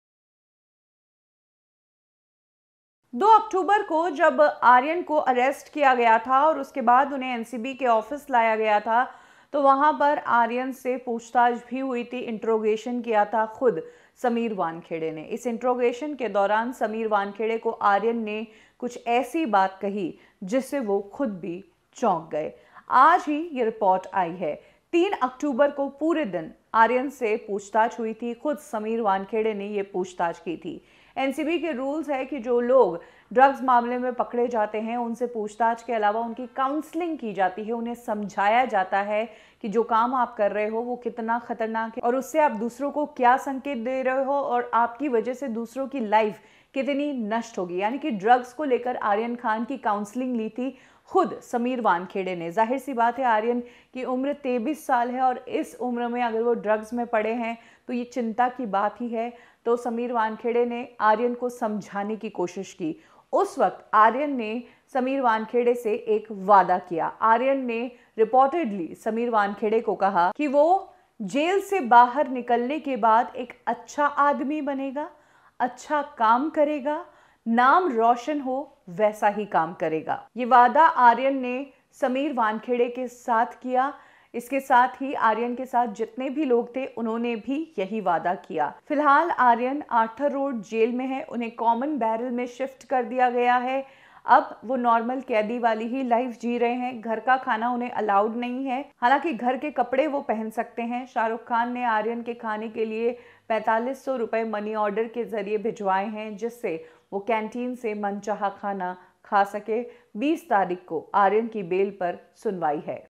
"क्या Future में नशा करोगे?" आँखों में आँखे डाल के जो जवाब दिया Aryan ने, उससे Sameer के होश उड़ गये
Big News | "क्या Future में नशा करोगे?" आँखों में आँखे डाल के जो जवाब दिया Aryan ने उससे Sameer के होश उड़ गये